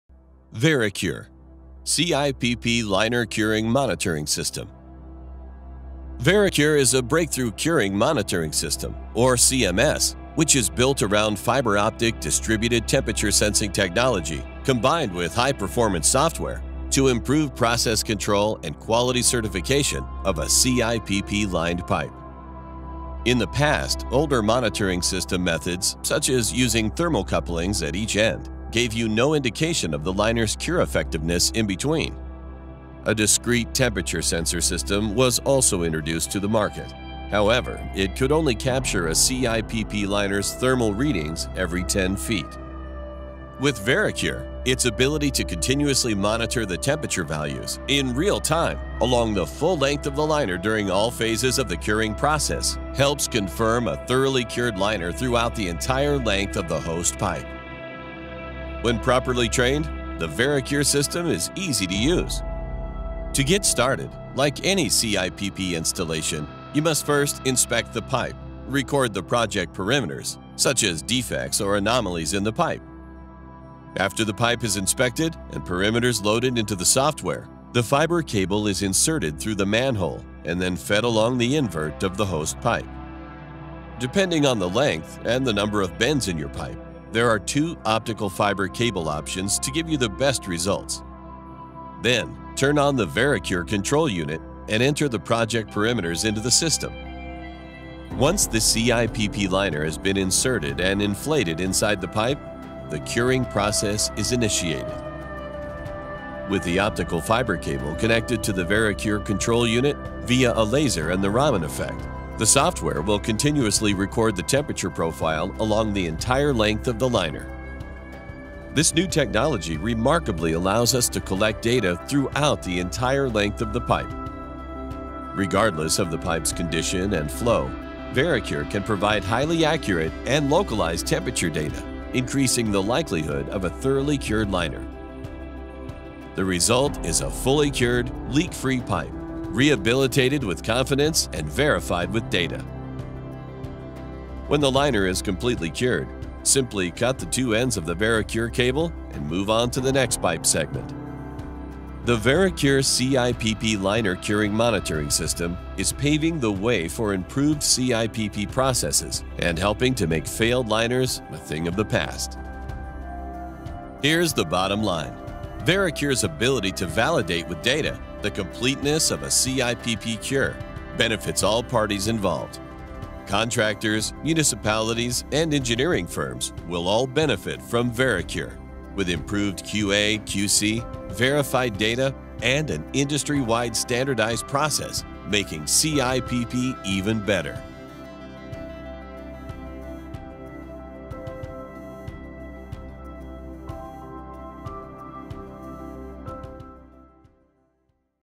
VeriCure® CIPP Curing Monitoring System
Cure CIPP liners with confidence using VeriCure technology.

Transcript:
VeriCure CIPP liner curing monitoring system.
VeriCure is a breakthrough curing monitoring system or CMS which is built around fiber optic distributed temperature sensing technology combined with high performance software to improve process control and quality certification of a CIPP lined pipe . In the past older monitoring system methods such as using thermal couplings at each end gave you no indication of the liner's cure effectiveness in between a discrete temperature sensor system was also introduced to the market however it could only capture a CIPP liner's thermal readings every 10 feet with VeriCure its ability to continuously monitor the temperature values in real time along the full length of the liner during all phases of the curing process helps confirm a thoroughly cured liner throughout the entire length of the host pipe when properly trained the VeriCure system is easy to use to get started like any CIPP installation you must first inspect the pipe record the project perimeters such as defects or anomalies in the pipe after the pipe is inspected and perimeters loaded into the software the fiber cable is inserted through the manhole and then fed along the invert of the hose pipe depending on the length and the number of bends in your pipe there are two optical fiber cable options to give you the best results then turn on the VeriCure control unit and enter the project perimeters into the system once the CIPP liner has been inserted and inflated inside the pipe the curing process is initiated with the optical fiber cable connected to the VeriCure control unit via a laser and the raman effect the software will continuously record the temperature profile along the entire length of the liner this new technology remarkably allows us to collect data throughout the entire length of the pipe regardless of the pipe's condition and flow VeriCure can provide highly accurate and localized temperature data increasing the likelihood of a thoroughly cured liner the result is a fully cured leak-free pipe rehabilitated with confidence and verified with data when the liner is completely cured simply cut the two ends of the VeriCure cable and move on to the next pipe segment the VeriCure CIPP liner curing monitoring system is paving the way for improved CIPP processes and helping to make failed liners a thing of the past here's the bottom line VeriCure's ability to validate with data the completeness of a CIPP cure benefits all parties involved contractors municipalities and engineering firms will all benefit from VeriCure with improved qaqc verified data and an industry-wide standardized process making CIPP even better.